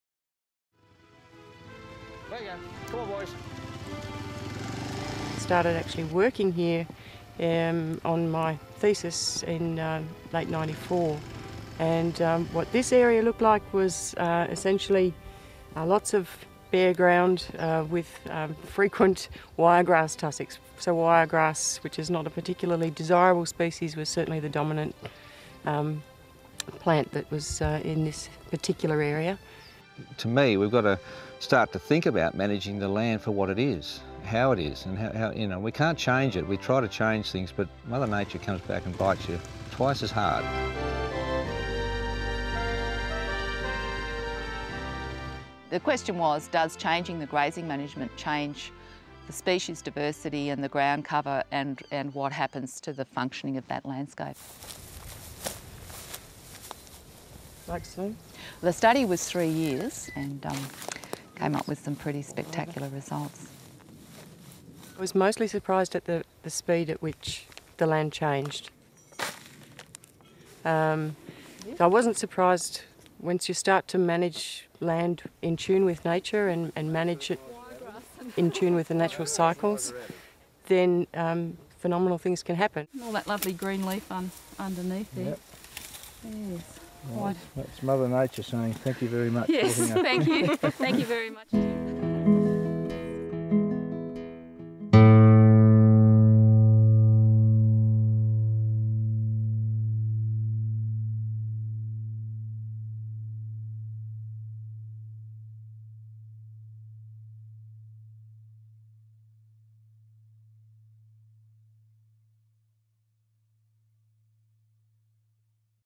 Holistic Management: "I was surprised at the speed at which the land changed..."
The question was, does changing the grazing management change the species diversity and the ground cover, and what happens to the functioning of that landscape? The study was three years and came out with some pretty spectacular results. I was mostly surprised at the speed at which the land changed... once you start to manage land in tune with nature, with the natural cycles, then phenomenal things can happen.